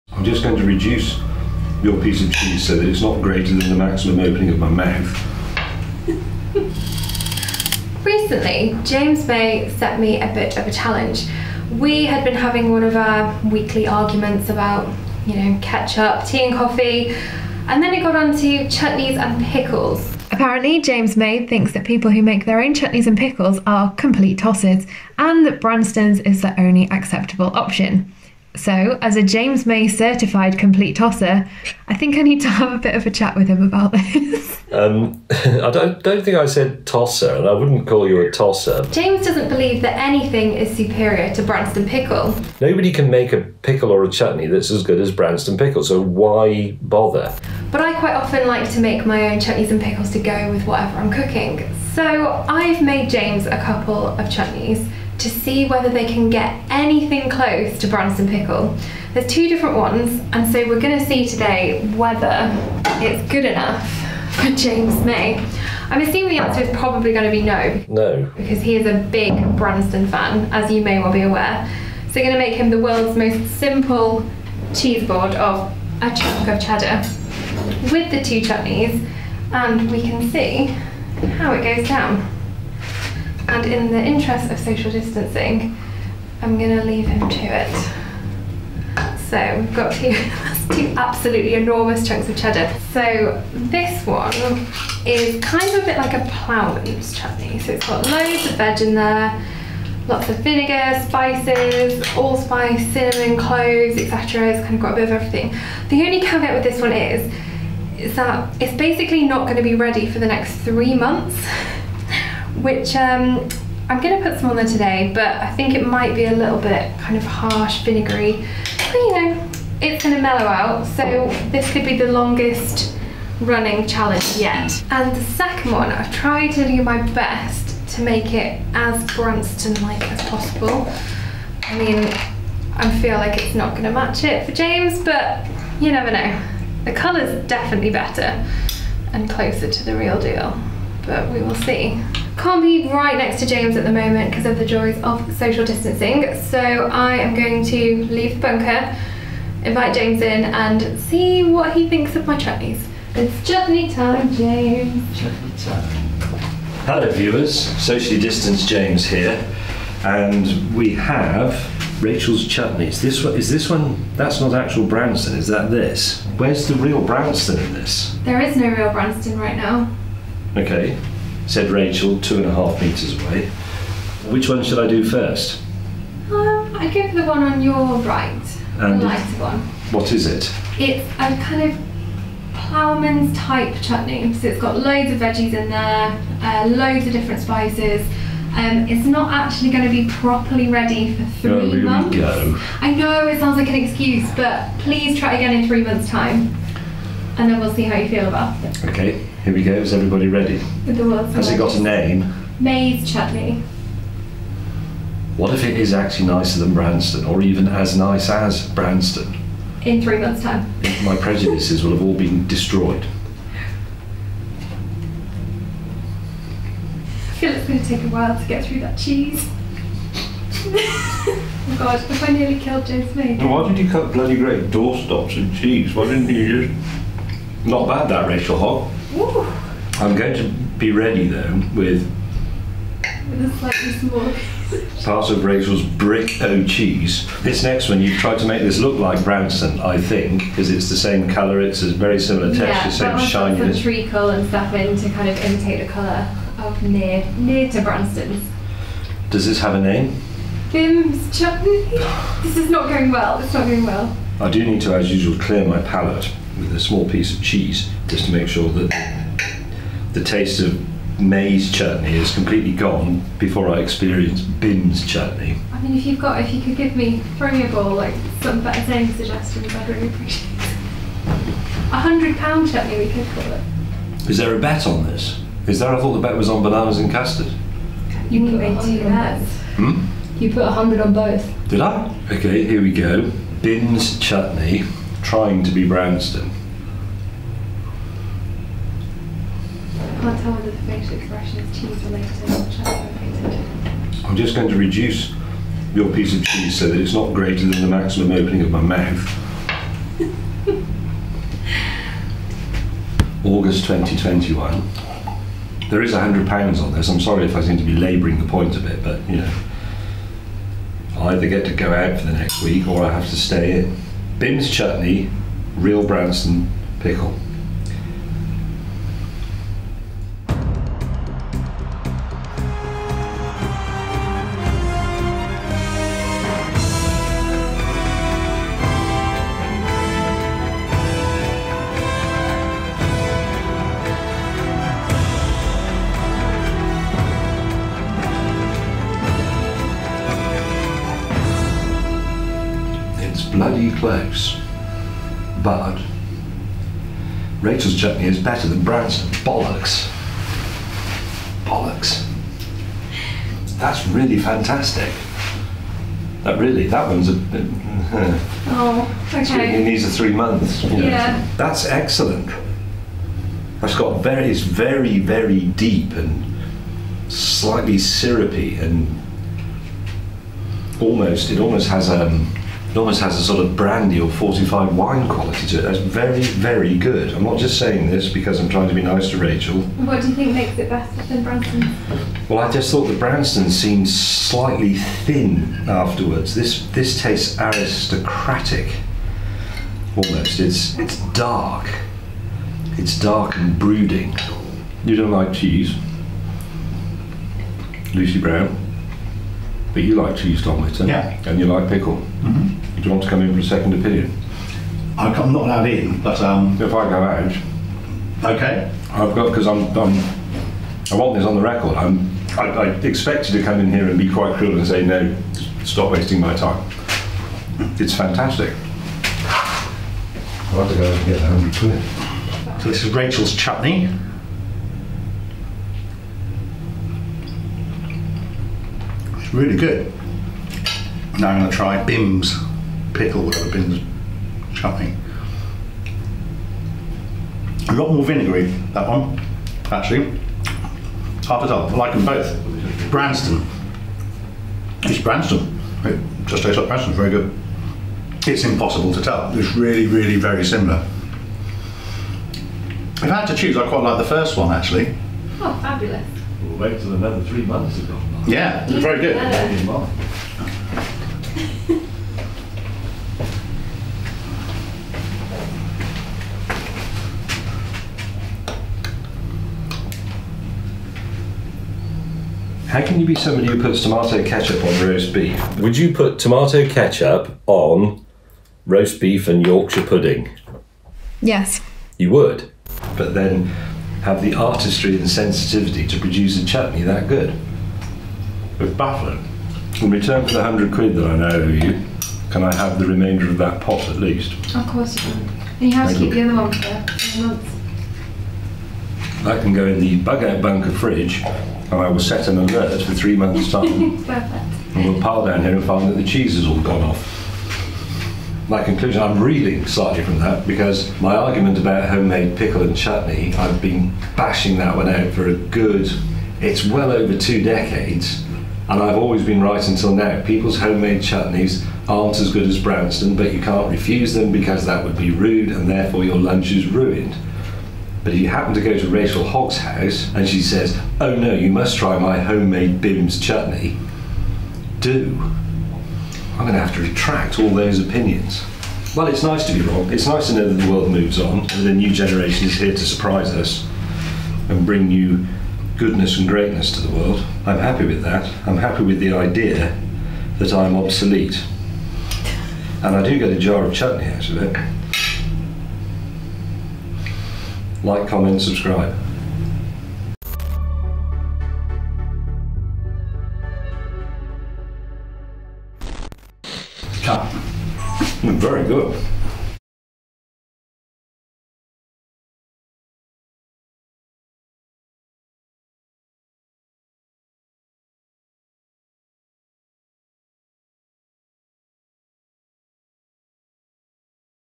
James May tries Rachael's homemade pickle
A few months ago, James May made the bold statement that NO pickle is as good as Branston Pickle. This week, in an attempt to prove him wrong, FoodTribe Editor Rachael Hogg has brought some homemade pickle to the bunker for him to try.